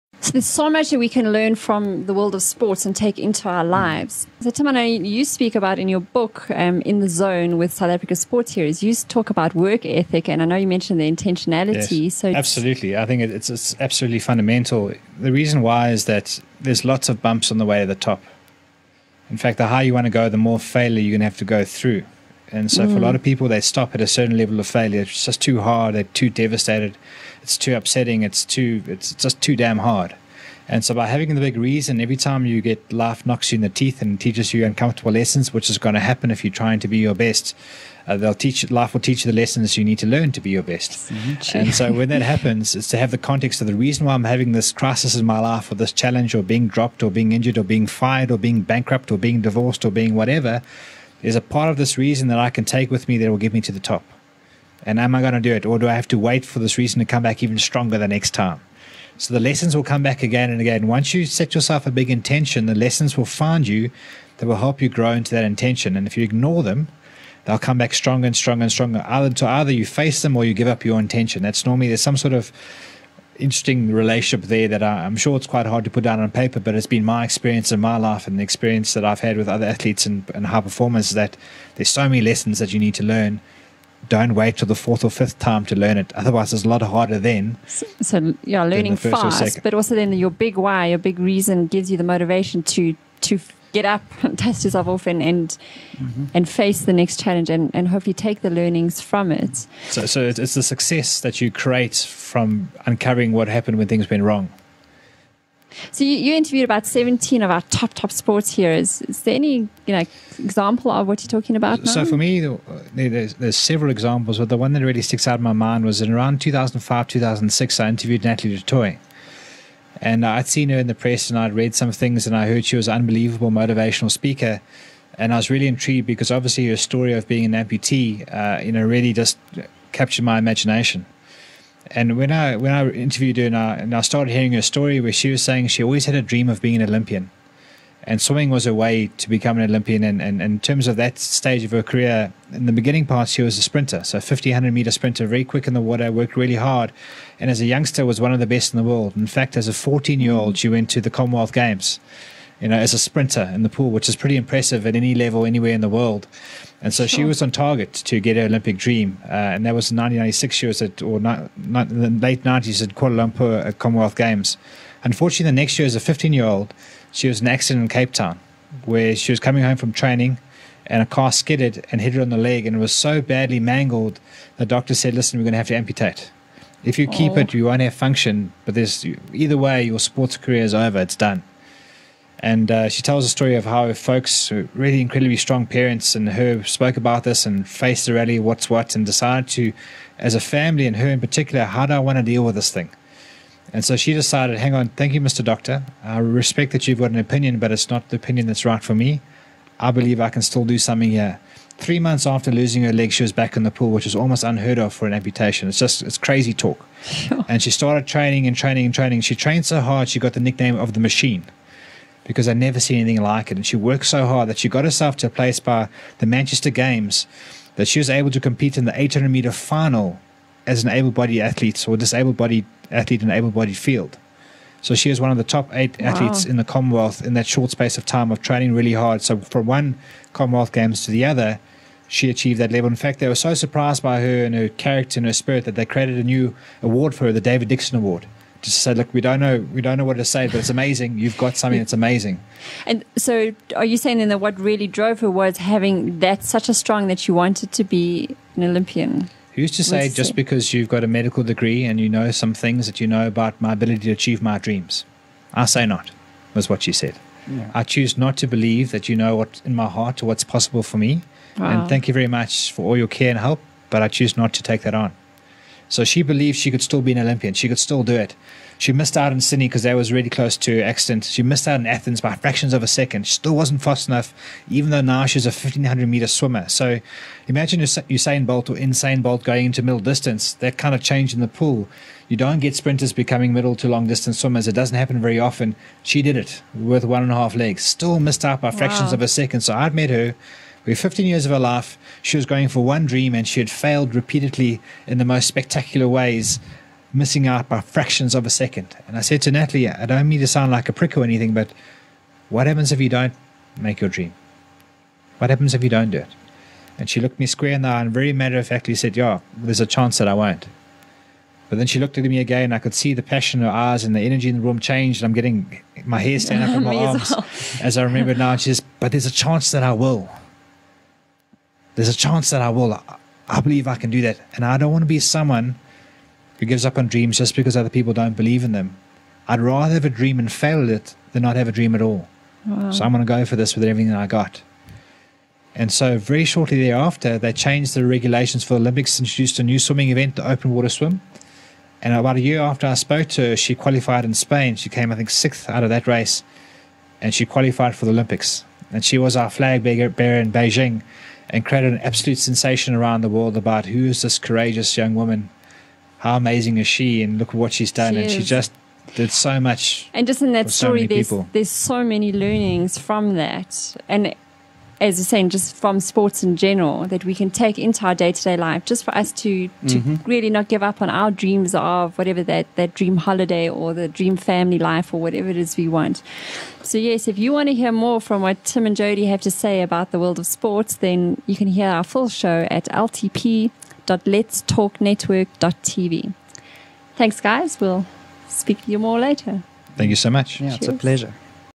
How having a big reason can get you over any hump - LTP E014
=In this show=
*Telana Simpson

=Guests=
*Tim Goodenough

=Topics=
*When we hear of a sports hero who has broken a record, or our favourite team wins a world cup event, we get so excited.  There is something about a person being able to perform at such a high level, that amazes us!
*With Telana is Tim Goodenough, a Meta Coach who specializes in working with Elite Athletes and sports teams, and who wrote the book 'In the Zone with South Africas Sports Heros' with co-author Michael Cooper. And Jody Martins, whos also a Meta- Coach and calls himself a Dream Coach, and who also has been coaching cricket for over a decade.
*Tim interviewed 17 of South Africas top athletes and analysed their mental skills, looking for what supported them to achieve such high levels of performance.
**Who were some of those athletes? (Graeme Smith, Jonty Rhodes, Penny Heyns, Nass Botha)
**What makes one athlete great and another merely good?
**How can athletes perform at the top of their game and do so consistently?
**Is it really all in the mind?
**And how can these principles be applied in business and life in general?

=How having a big reason can get you over any hump=